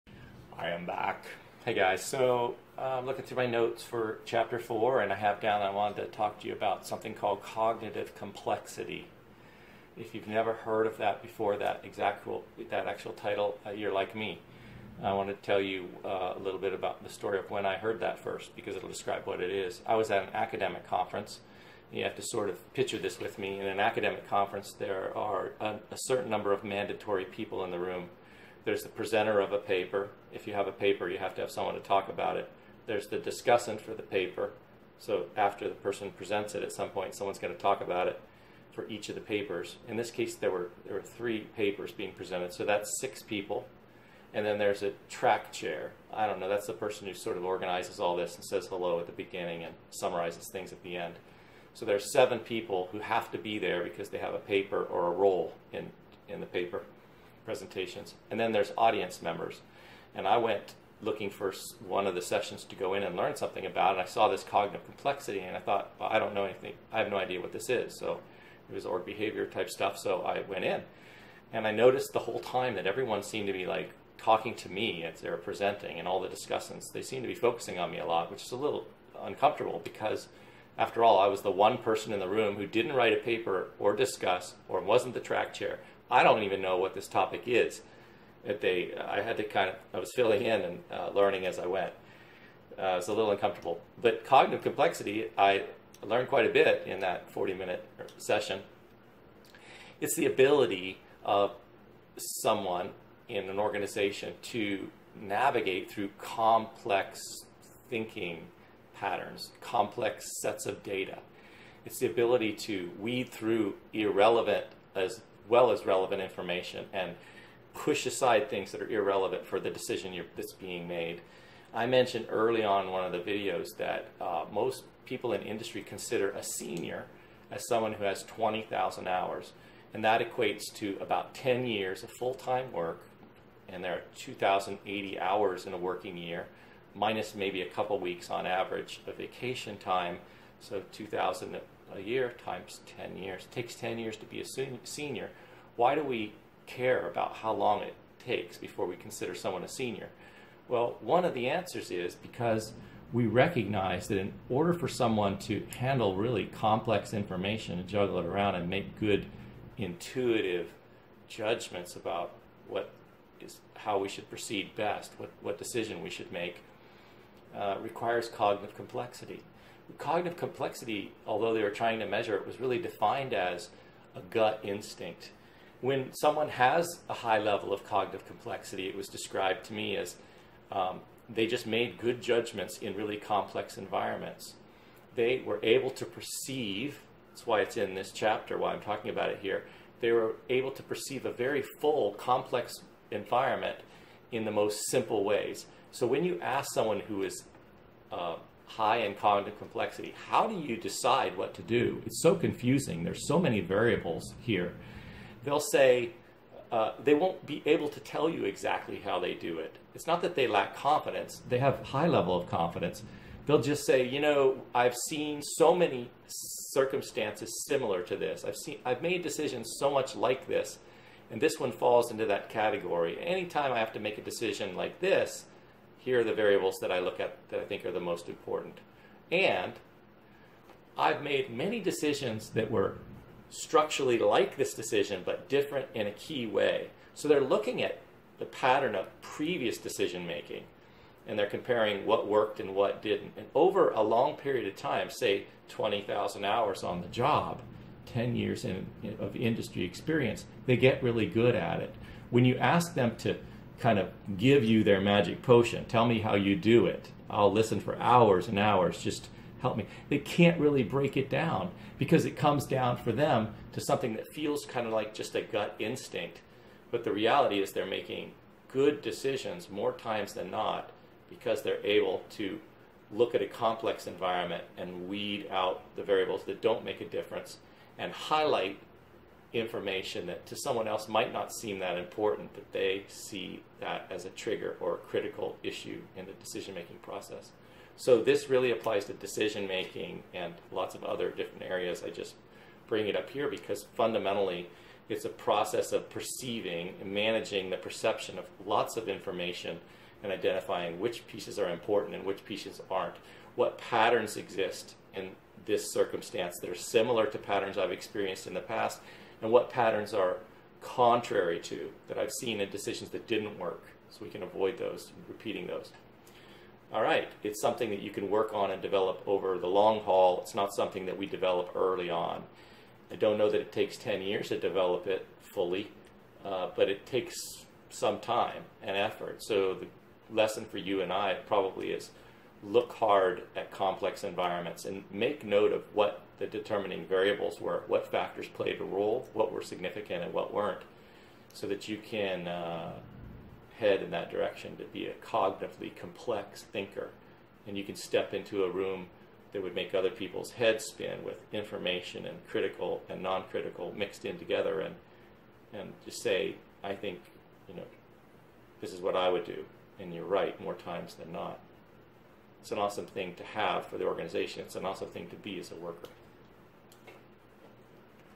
Cognitive Complexity
A description of cognitive complexity, what it is, how it works, and how to build it through experience.